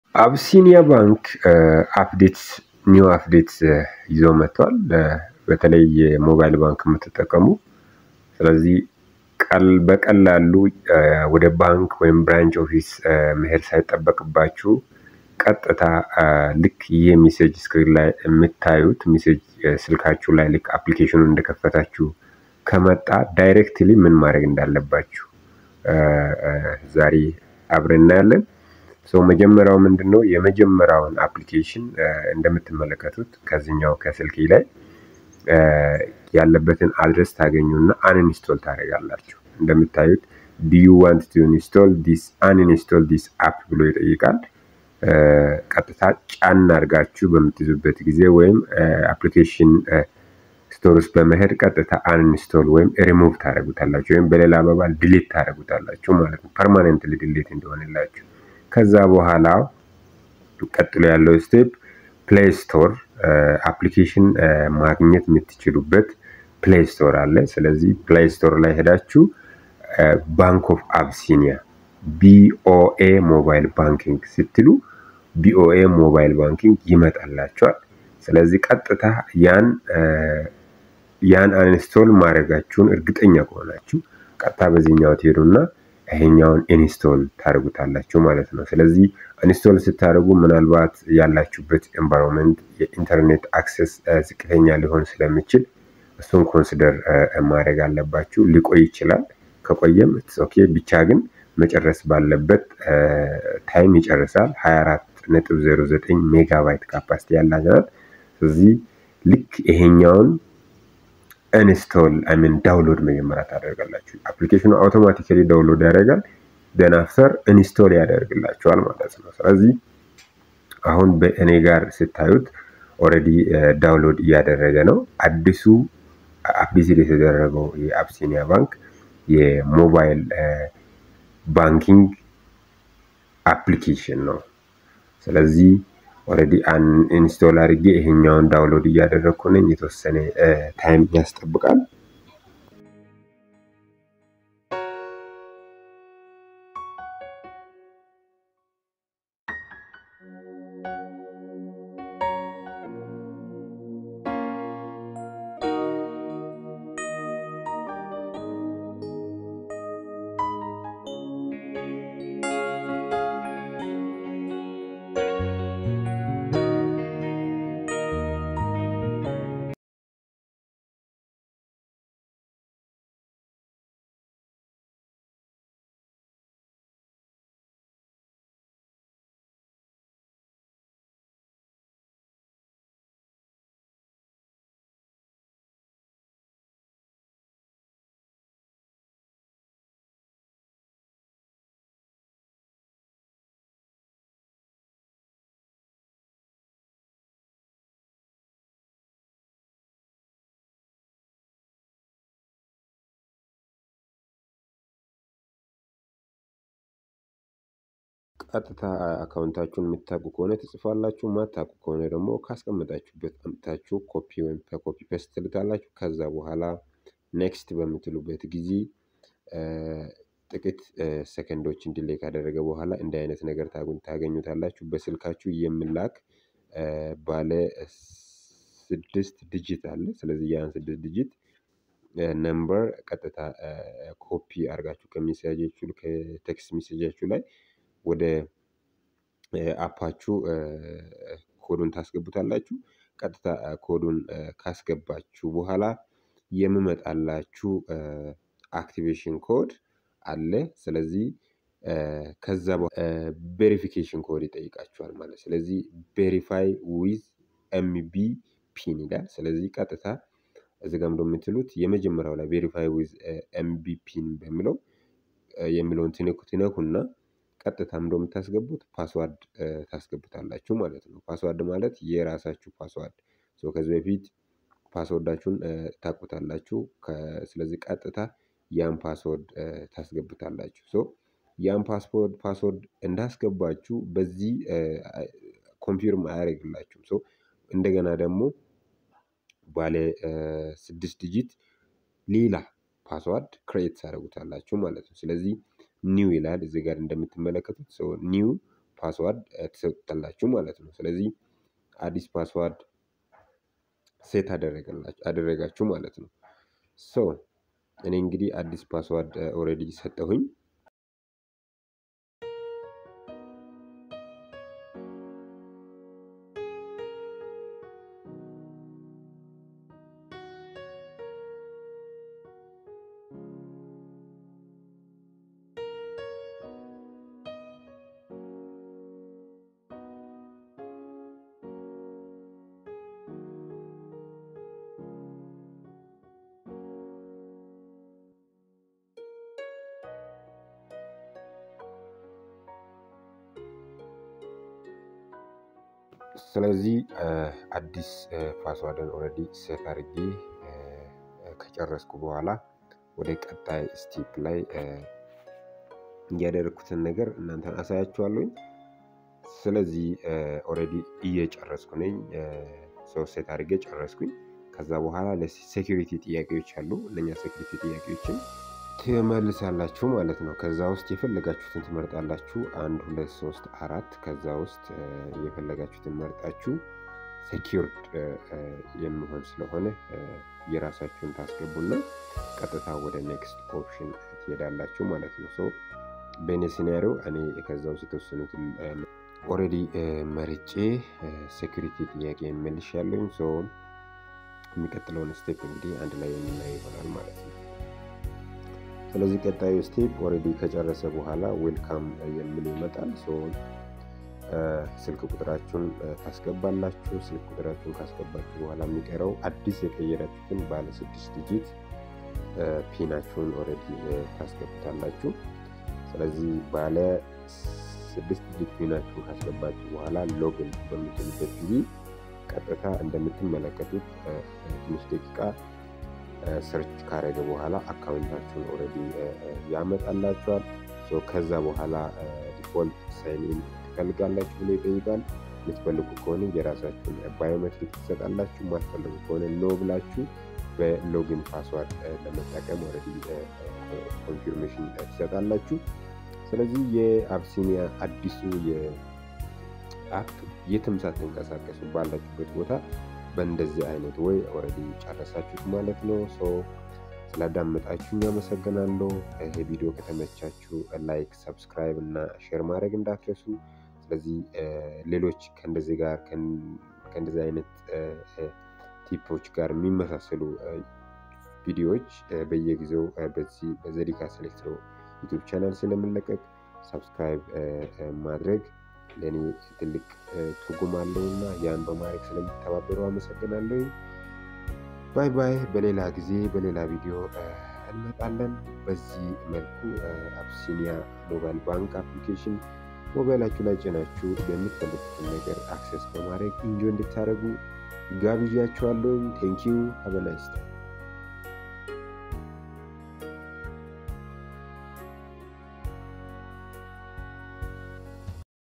how to Activate BOA mobile banking|Bank of Abyssiniya mobile banking
this is how to activate the updates BOA MOBILE banking at home with out going to branch office for support
0:00 intro
1:00 uninstallation of old app
1:55 playstore
4:00 download and installation
6:00 password confirm